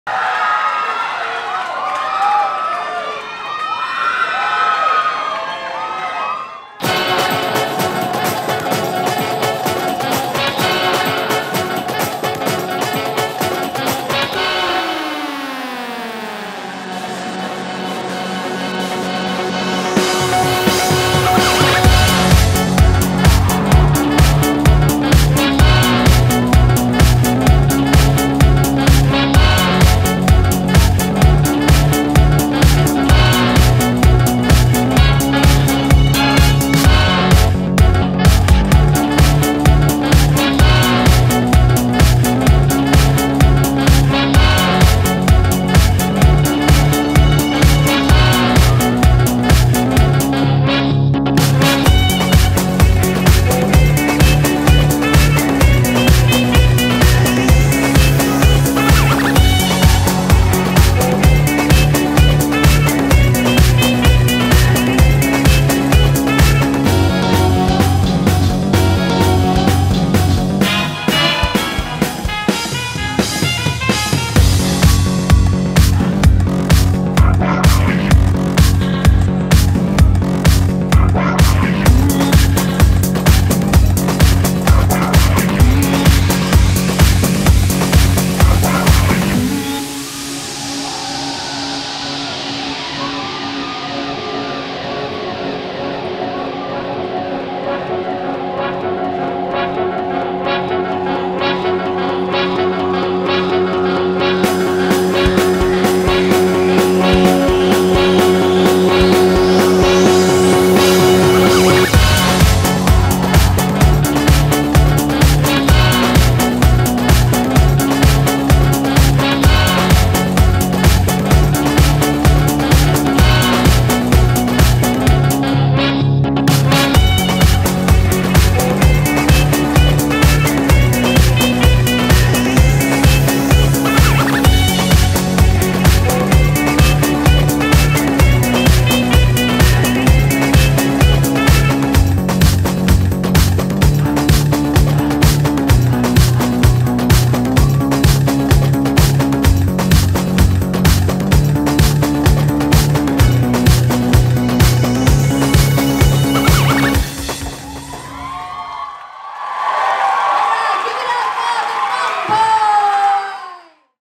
[WIDE VIEW] Fu*k Boys | Open Division | WSB Singapore 2018
WSB Singapore seeks to empower and motivate the Singapore dance community. This platform gives dancers a chance to battle it out for the title and a chance to represent at the WSB International Championship in Australia.